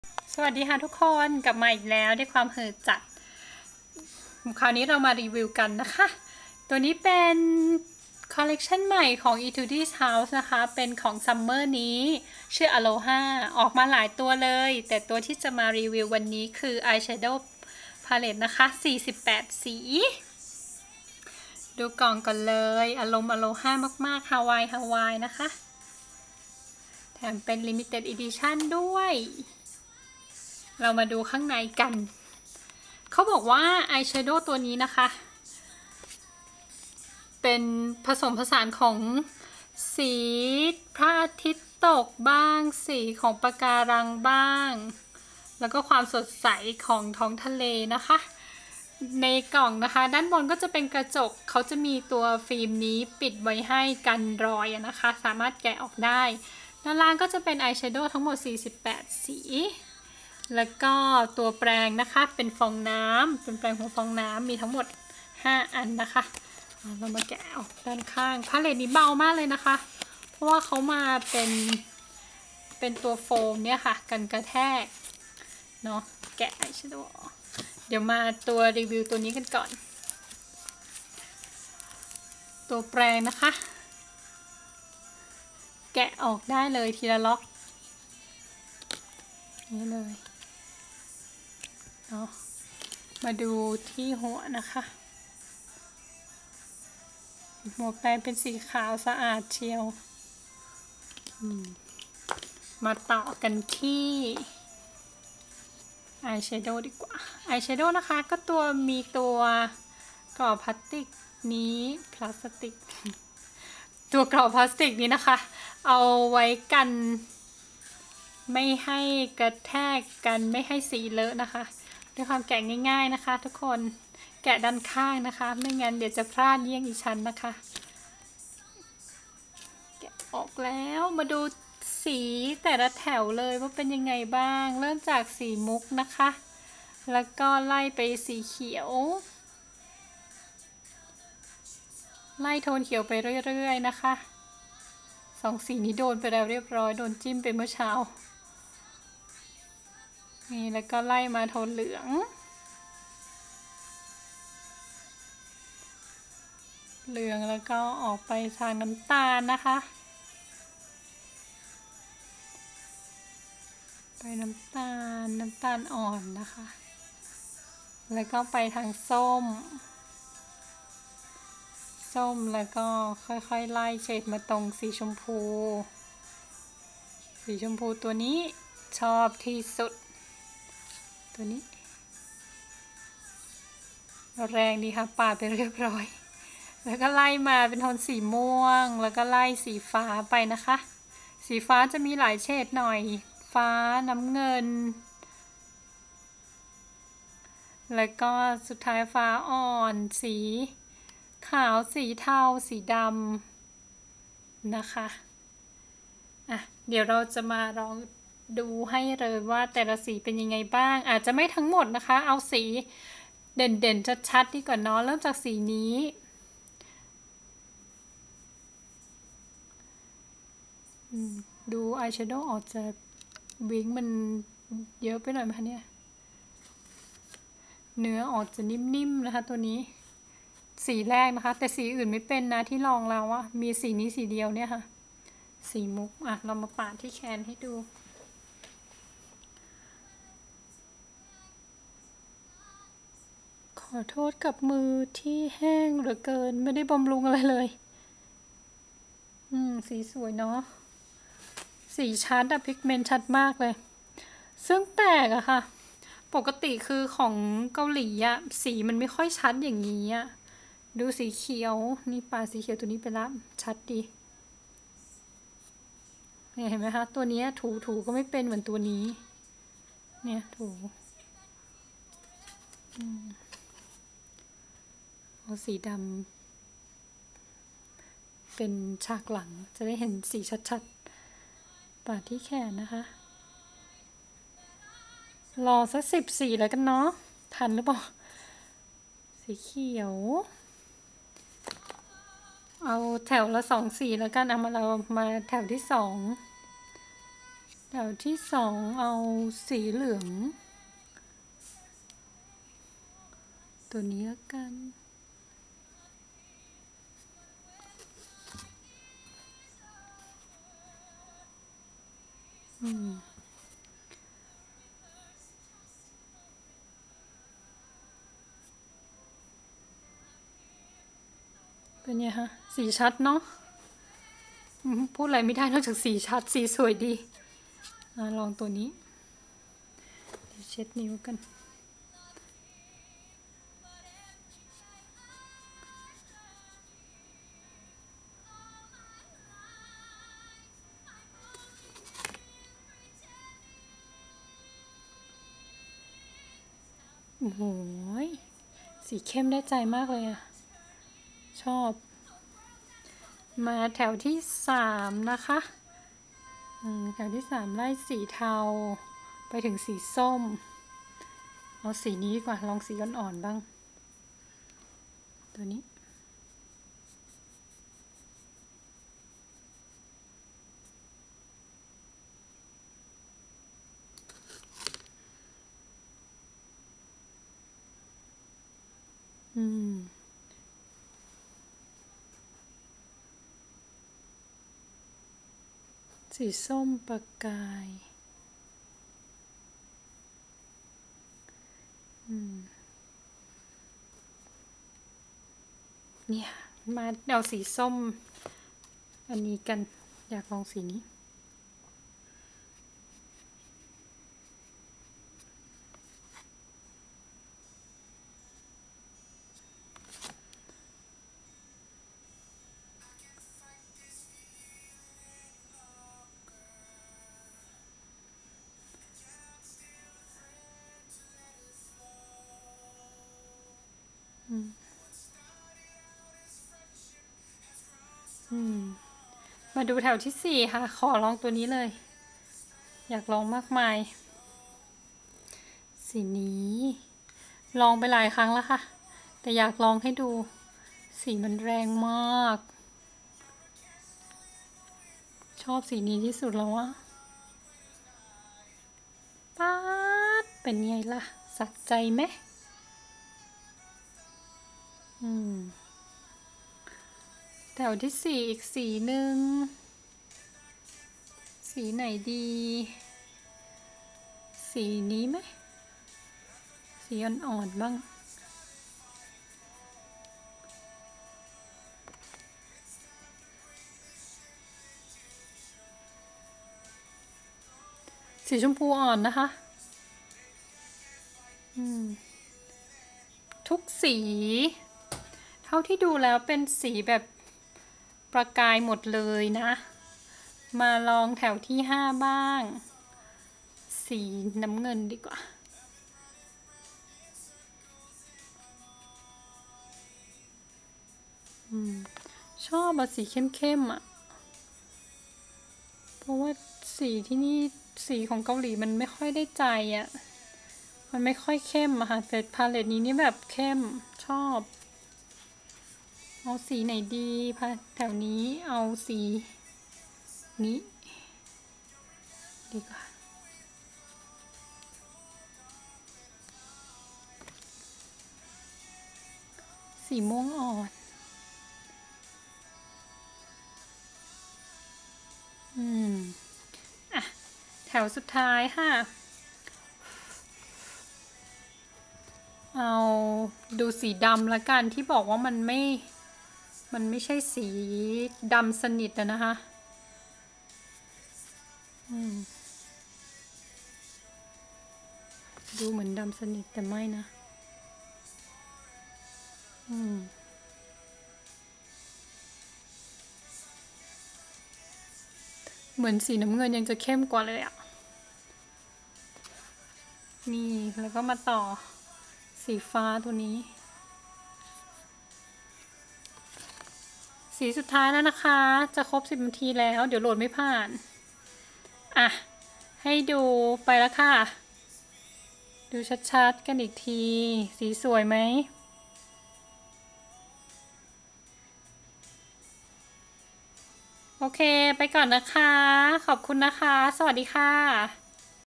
Molly_Nanny : Etude House - Aloha 48 Color of Tropical Eyes [Review]
Etude House New Summer Collection 2011 : "Aloha"

Review : Aloha 48 colors of Tropical Eyes
Price : 20,000 won